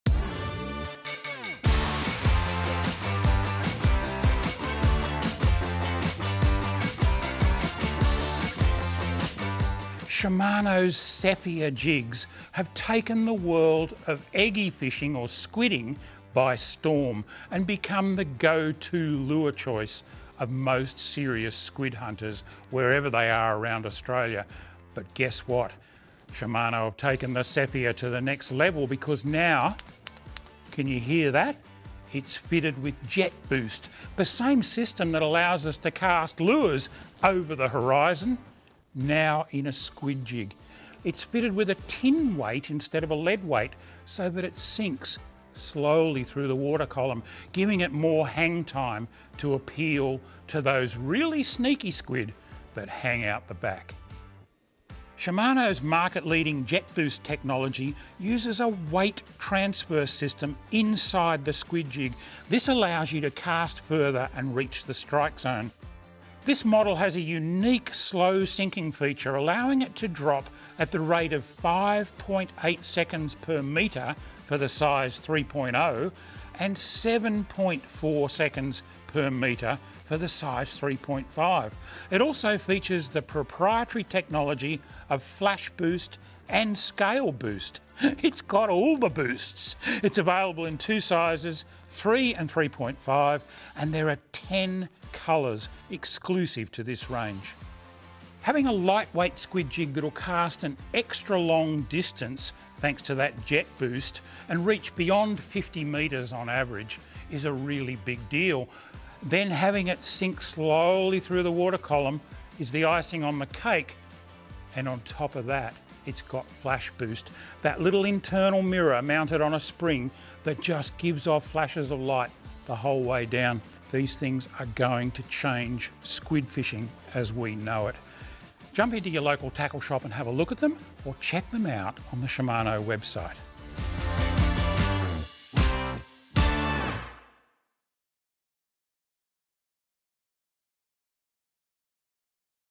SHIMANO TACKLE TOUR 2023: Sephia Clinch Long Appeal
The world of squid fishing will be transformed by the remarkable casting distance and slow fall capabilities of the Sephia Clinch Long Appeal!

Overcoming the limitations of traditional shallow-type lures, this innovative lure features Shimano's JetBoost mechanism for unprecedented casting performance, reaching an average distance of 52 metres. With a slower sinking speed and patented FlashBoost technology to attract squid, the Sephia Clinch Long Appeal 3.5 gou opens new possibilities for targeting large and cautious squid.